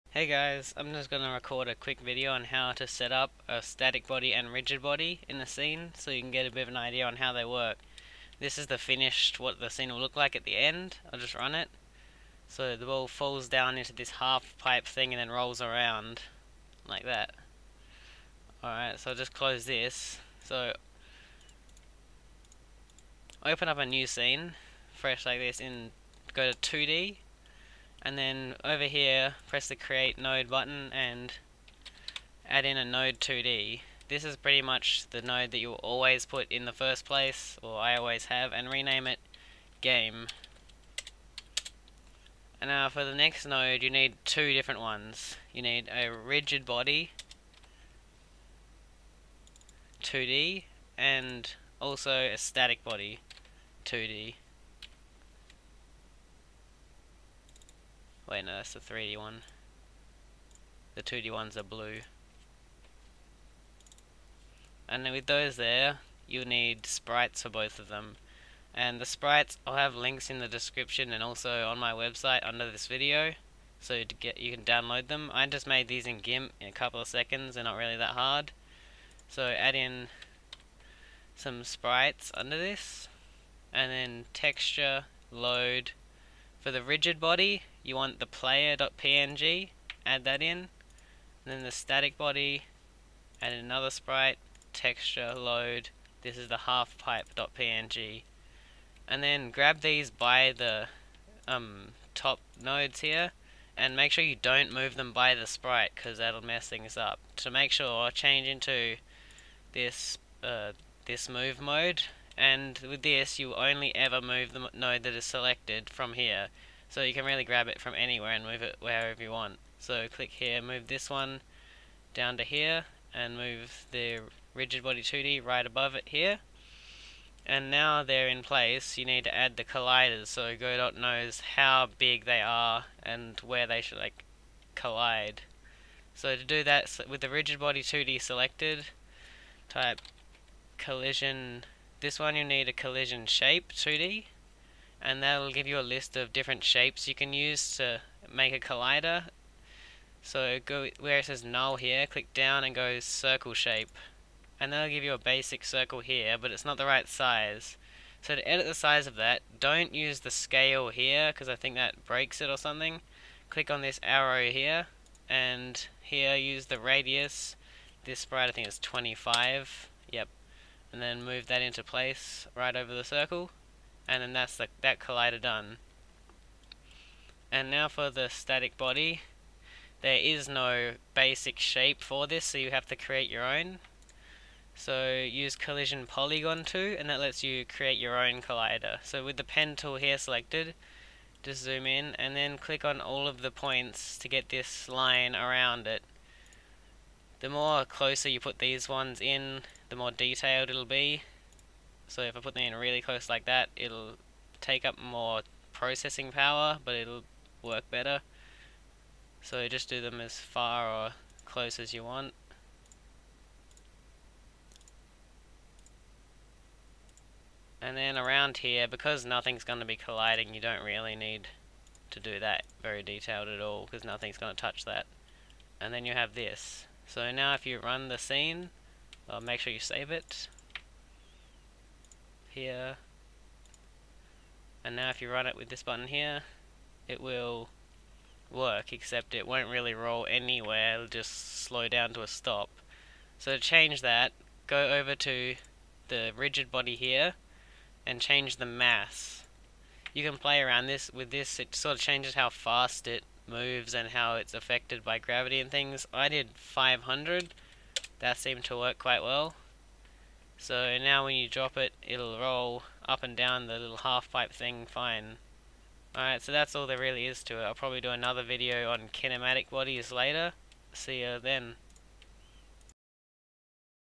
Rigid and static bodies in godot.
How to use rigid and static bodies in godot. Sorry about the sidebar glitching out I need to find out whats doing that..